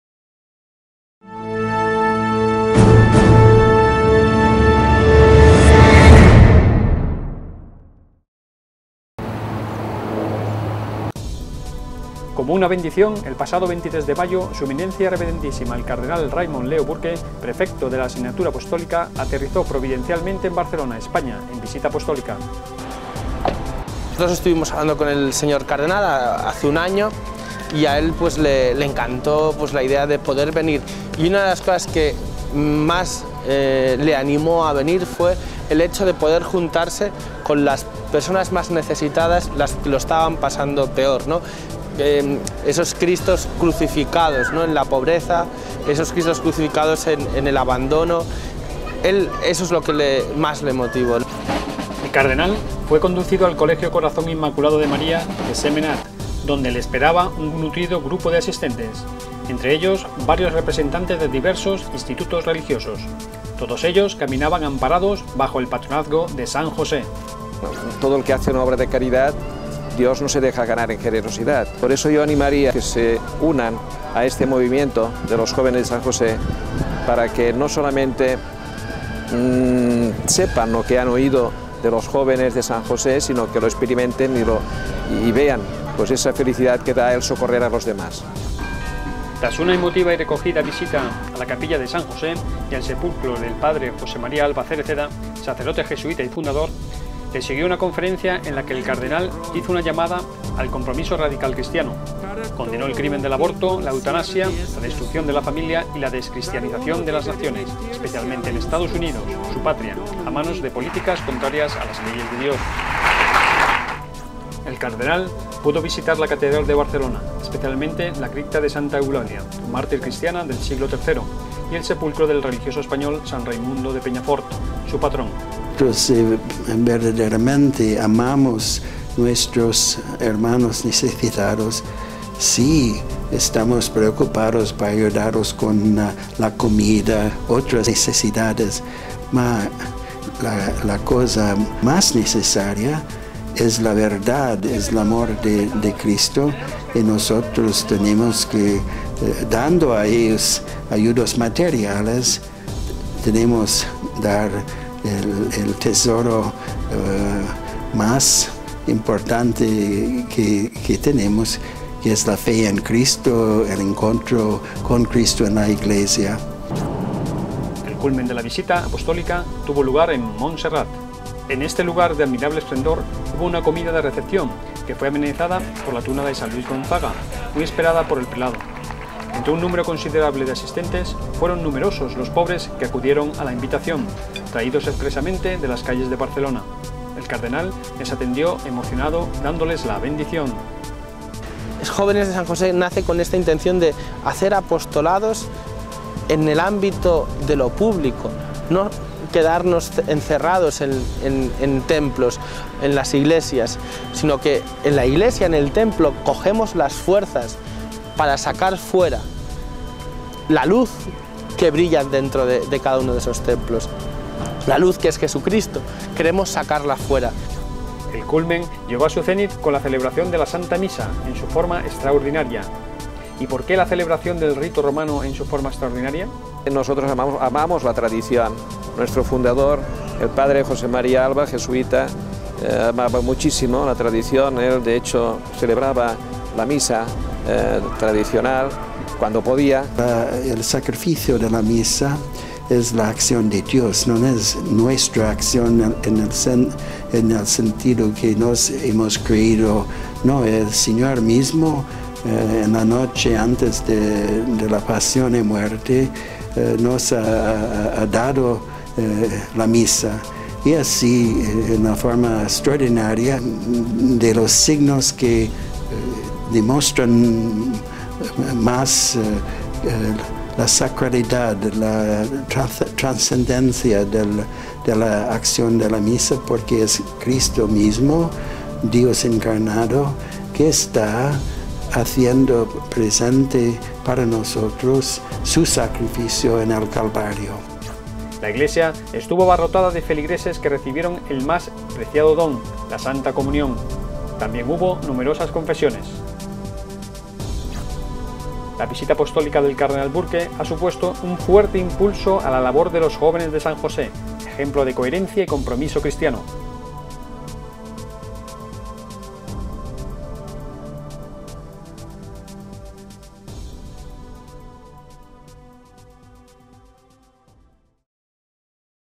NOTICIA Visita Apostolica S.E.R. Cardenal Burke a España
Noticia de la visita de S.E.R. Cardenal Raymond Leo Burke a Barcelona. 2014